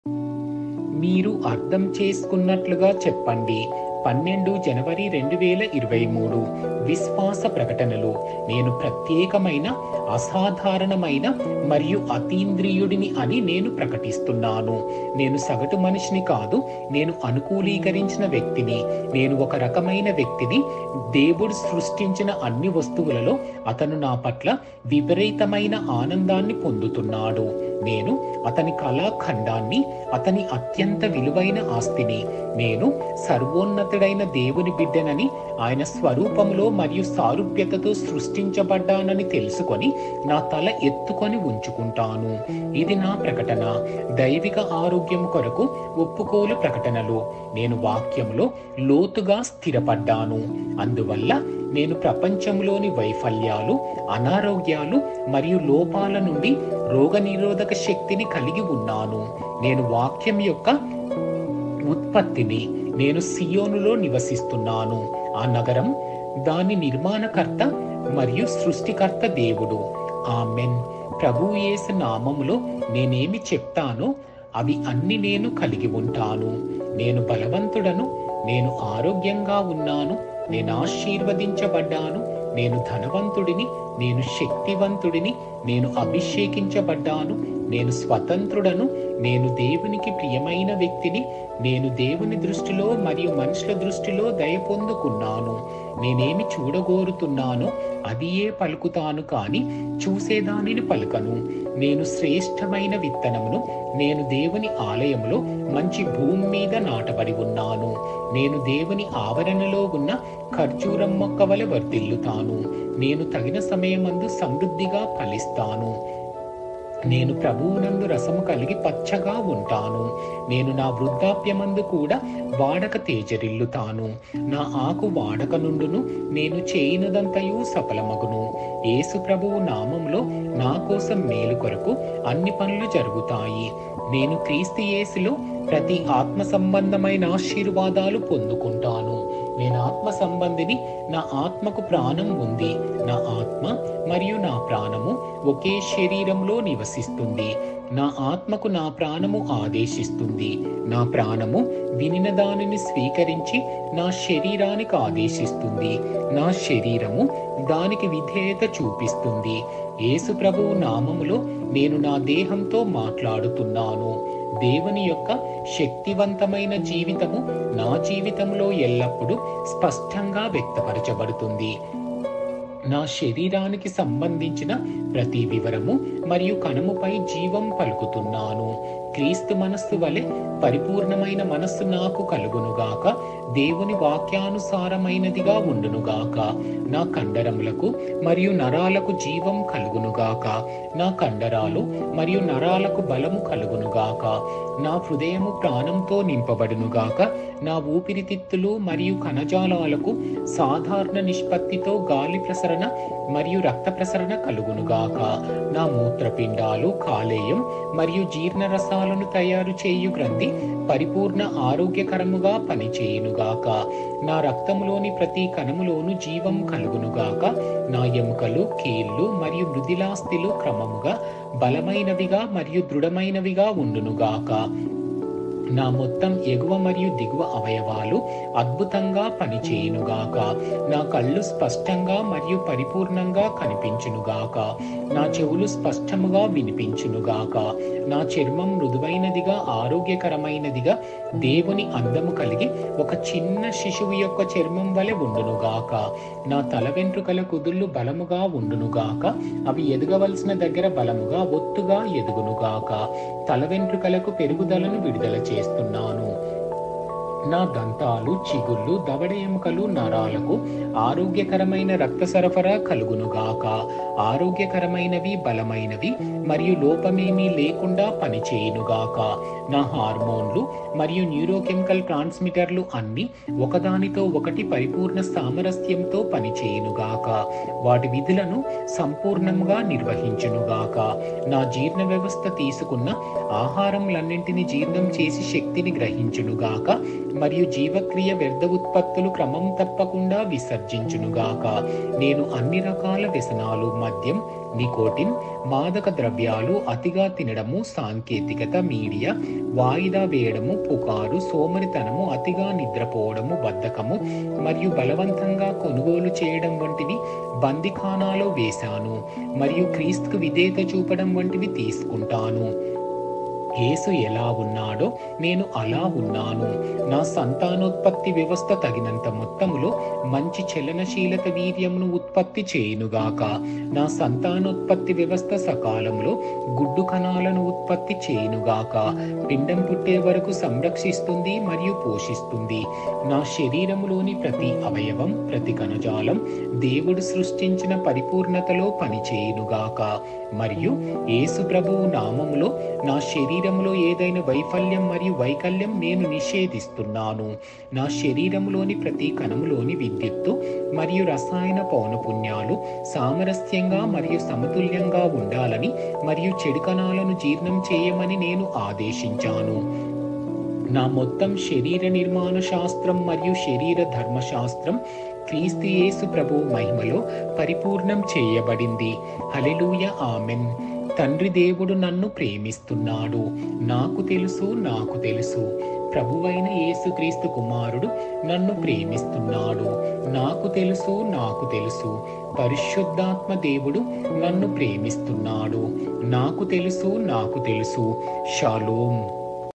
12th January 2023 Say It like You Mean It (Audio in Telugu)
*మీరు అర్థం చేసుకున్నట్లుగా చెప్పండి*

*12 జనవరి 2023*

*విశ్వాస ప్రకటనలు*

నేను ప్రత్యేకమైన, అసాధారణమైన మరియు అతీంద్రియుడిని అని నేను ప్రకటిస్తున్నాను. నేను సగటు మనిషిని కాదు! నేను అనుకూలీకరించిన వ్యక్తిని. నేను ఒక రకమైన వ్యక్తిని. దేవుడు సృష్టించిన అన్ని వస్తువులలో, అతను నా పట్ల విపరీతమైన ఆనందాన్ని పొందుతున్నాడు* నేను అతని కళాఖండాన్ని, అతని అత్యంత విలువైన ఆస్తిని.* నేను సర్వోన్నతుడైన దేవుని బిడ్డనని, ఆయన స్వరూపంలో మరియు సారూప్యతతో సృష్టించబడ్డానని తెలుసుకుని, నా తల ఎత్తుకుని ఉంచుకుంటాను. ఇది నా ప్రకటన.

* దైవిక ఆరోగ్యము కొరకు ఒప్పుకోలు ప్రకటనలు*

నేను వాక్యంలో లోతుగా స్థిరపడ్డాను; అందువల్ల, నేను ప్రపంచంలోని వైఫల్యాలు, అనారోగ్యాలు మరియు లోపాల నుండి రోగనిరోధక శక్తిని కలిగి ఉన్నాను. *నేను వాక్యం యొక్క ఉత్పత్తిని. నేను సీయోనులో నివసిస్తున్నాను, ఆ నగరం దీని నిర్మాణకర్త మరియు సృష్టికర్త దేవుడు. ఆమేన్*!

*ప్రభువు యేసు నామములో నేను ఏమి చెప్తానో అవి అన్ని నేను  కలిగి ఉంటాను (యోహాను 14:13)*

• నేను బలవంతుడను (ఎఫెసీయులకు 6:10)
• నేను ఆరోగ్యంగా ఉన్నాను (1 పేతురు 2:24)
• నేను ఆశీర్వదించబడ్డాను (ఎఫెసీయులకు 1:3)
• నేను ధనవంతుడిని (2 కొరింథీయులు 8:9)
• నేను శక్తివంతుడిని (యెషయా 40:29)
• నేను అభిషేకించబడ్డాను (1 యోహాను 2:20)
• నేను స్వతంత్రుడను (యోహాను 8:36)
• నేను దేవునికి ప్రియమైన వ్యక్తిని (మార్కు 1:11)
• నేను దేవుని దృష్టిలో మరియు మనుషుల దృష్టిలో దయ పొందుకున్నాను (ఆదికాండము 39:2-3)
• నేను ఏమి చూడగోరుతున్నానో అదియే పలుకుతాను , కానీ చూసేదానిని పలుకను (2కొరింథీయులు 4:18)
• నేను శ్రేష్ట్రమైన విత్తనంను (లూకా 8:15)
• నేను దేవుని ఆలయంలో మంచి భూమిమీద నాటబడి ఉన్నాను (కీర్తన 92:13)
• నేను దేవుని ఆవరణలో ఉన్నం ఖర్జూరం మొక్కవలె వర్ధిల్లుతాను (కీర్తన 92:13)
• నేను తగిన సమయమందు సమృద్ధిగా ఫలిస్తాను (కీర్తన 1:3)
• నేను ప్రభువునందు రసం కలిగి పచ్చగా ఉంటాను (కీర్తన 92:14)
• నేను నా వృదాప్యంఅందు కూడా వాడక  తేజరిల్లుతాను (కీర్తన s 92:14)
• నా  ఆకు వాడక నుండును నేను చేయునదంతయు సఫలమగును. (కీర్తన 1:3)
• యేసు ప్రభువు నామంలో నా కోసం మేలు కొరకు అన్ని పనులు జరుగుతాయి. (రోమీయులకు 8:28)
• నేను క్రీస్తు యేసులో ప్రతి ఆత్మ సంబంధమైన ఆశీర్వాదాలు  పొందుకుంటాను . (ఎఫెసీయులకు 1:3)

• నేను ఆత్మ సంబంధిని
• నా ఆత్మకు ప్రాణము ఉంది
• నా ఆత్మ మరియు నా ప్రాణము ఓకే శరీరంలో నివసిస్తుంది
• నా ఆత్మకు నా ప్రాణము ఆదేశిస్తుంది.  నా ప్రాణము వినినదానిని స్వీకరించి నా శరీరానికి ఆదేశిస్తుంది, నా శరీరము దానికి విధేయత చూపిస్తుంది .

*యేసు ప్రభువు నామంలో, నేను నా దేహంతో మాట్లాడుతున్నాను*

• దేవుని యొక్క శక్తి వంతమైన జీవితము, నా జీవితంలో ఎల్లప్పుడూ స్పష్టంగా వ్యక్తపరచబడుతుంది .
• నా శరీరానికి సంబంధించిన ప్రతి వివరము/ కణము పై జీవము పలుకుతున్నాను
✝️క్రీస్తు మనస్సు వలె పరిపూర్ణంమైన  మనస్సు  నాకు కలుగును గాక, దేవుని వాక్యానుసారమైనదిగా ఉండును గాక .
✝️నా కండరమూలకు మరియు  నరాలకు జీవము కలుగును గాక
✝️నా కండరాలు మరియు నరాలకు బలము కలుగును గాక
✝️నా హృదయము ప్రాణముతో నింపబడును గాక
✝️నా ఊపిరితిత్తులు మరియు కణజాలాలకు సాధారణ నిష్పత్తితో గాలి/వాయు ప్రసరణ మరియు రక్త ప్రసరణ కలుగునుగాక.
✝️నా మూత్రపిండాలు, కాలేయం మరియు జీర్ణరసాలను తయారుచేయు గ్రంథి పరిపూర్ణ  ఆరోగ్యకరముగా  పని చేయునుగాక
✝️నా రక్తంలోని ప్రతి కణంలోనూ జీవము కలుగును గాక
✝️నా ఎముకలు, కీళ్ళు మరియు మృదులాస్థిలు క్రమముగా, బలమైనవిగ మరియు దృఢమైనవిగ ఉండును గాక
✝️నా మొత్తం ఎగువ మరియు దిగువ అవయవాలు అద్భుతంగా పని చేయునుగాక
✝️నా కళ్ళు స్పష్టంగా మరియు పరిపూర్ణంగా కనిపించును గాక
✝️నా చెవులు స్పష్టముగా వినిపించును గాక
✝️నా చర్మం మృదువైనదిగ, ఆరోగ్యకరమైనదిగా, దేవుని అందం కలిగి ఒక చిన్న శిశువు యొక్క చర్మం వలె ఉండును గాక
✝️నా తల వెంట్రుకల కుదుళ్లు బలముగా ఉండును గాక,  అవి ఎదగవలసిన దగ్గర బలముగా ఒత్తుగా ఎదుగును గాక, తల వెంట్రుకలకు పెరుగుదలను విడుదల చేస్తున్నాను
✝️నా దంతాలు, చిగుళ్ళు, దవడ ఎముకలు, నరాలకు ఆరోగ్యకరమైన రక్త సరఫరా కలుగును గాక,  ఆరోగ్యకరమైనవి, బలమైనవి మరియు లోపంఏమిలేకుండా పనిచేయును గాక.
✝️నా హార్మోన్లు మరియు న్యూరోకెమికల్ ట్రాన్స్‌మిటర్లు అన్నీ ఒకదానితో ఒకటి పరిపూర్ణ సామరస్యంతో పని చేయును గాక, వాటి విధులను సంపూర్ణముగా నిర్వహించును గాక
✝️నా జీర్ణవ్యవస్థ తీసుకున్న ఆహారములన్నింటినీ జీర్ణం చేసి, శక్తి ని గ్రహించును గాక మరియు జీవక్రియ వ్యర్థ ఉత్పత్తులుక్రమం తప్పకుండా విసర్జించును గాక
✝️నేను అన్ని రకాల *వ్యసనాలు (మద్యం, నికోటిన్, మాదకద్రవ్యాలు)*, *అతిగా తినడం, సాంకేతికత, మీడియా, వాయిదా వేయడం, పుకారు, సోమరితనం, అతిగా నిద్రపోవడం, బద్ధకం మరియు బలవంతంగా కొనుగోలు చేయడం వంటివి* బందిఖానాలో వేసాను మరియు క్రీస్తుకు విధేయత చూపడం వంటివి తీసుకుంటాను. యేసు ఎలా ఉన్నాడో నేను అలా ఉన్నాను*
✝️🙎🏻‍♂️ ♂️ నా సంతానోత్పత్తి వ్యవస్థ తగినంత మొత్తంలో మంచి చలనశీలత వీర్యమును ఉత్పత్తి చేయును గాక
✝️🙍🏻‍♀️ ♀️ నా  సంతానోత్పత్తి వ్యవస్థ సకాలంలో గుడ్డు కణాలను ఉత్పత్తి చేయును గాక, పిండం పుట్టే వరకు సంరక్షిస్తుంది మరియు పోషిస్తుంది *
✝️ నా శరీరంలోని ప్రతి అవయవం, ప్రతి కణజాలం దేవుడు సృష్టించిన పరిపూర్ణతలో పనిచేయును గాక మరియు యేసుప్రభువు నామంలో నా శరీరంలో ఏదైనా వైఫల్యం మరియు వైకల్యం నేను నిషేధిస్తున్నాను.
✝️నా శరీరంలోని ప్రతి కణంలోని *విద్యుత్తు మరియు రసాయన పౌనఃపున్యాలు* సామరస్యంగా మరియు సమతుల్యంగా ఉండాలని మరియు చెడు కణాలను జీర్ణం చేయమని నేను ఆదేశించాను